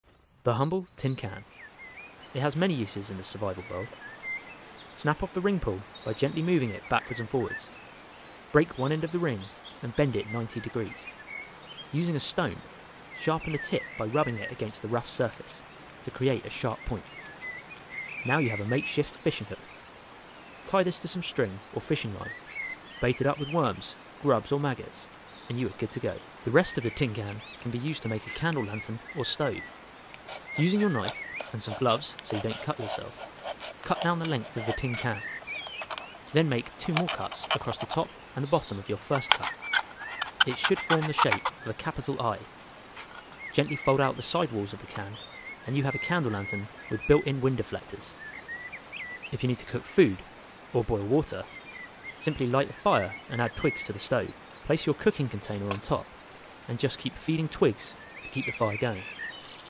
A Tin Can: Turn Trash Into a DIY Survival Gear
How to make a simple but effective stove and a fishing hook from a tin can?
These are great survival skills that can come in handy in an emergency. Watch and learn how to turn trash into treasure!

Also, make sure to visit the TA Outdoors channel on YouTube for more amazing content like this!